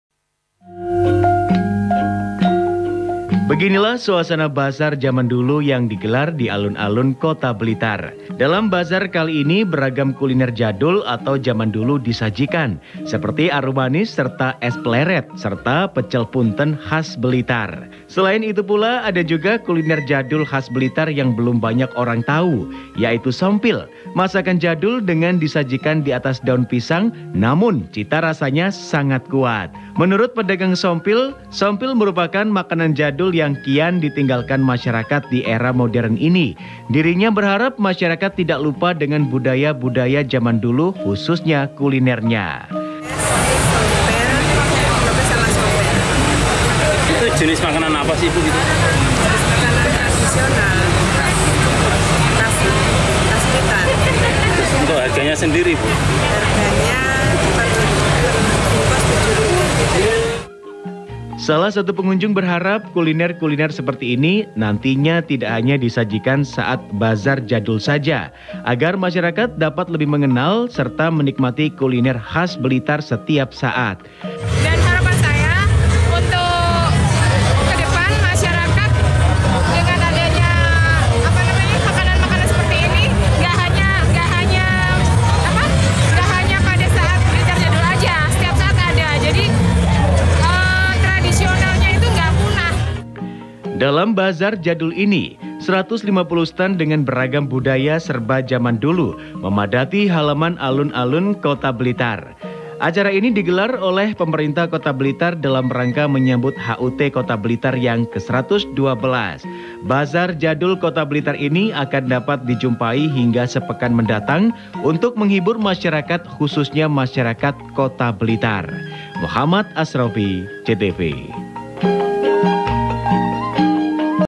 Beragam Kuliner Jadul Ala Blitar Disajikan Dalam Bazar Jaman Dulu
Jika anda ingin bernostalgia dengan keluner khas jaman dulu, ada baiknya anda mendatangi Kota Blitar. Beragam kuliner jaman dulu khas Kota Blitar seperti es pleret, lopis, serta pecel punten disajikan dalam bazar jadul yang digelar oleh Pemerintah Kota Blitar.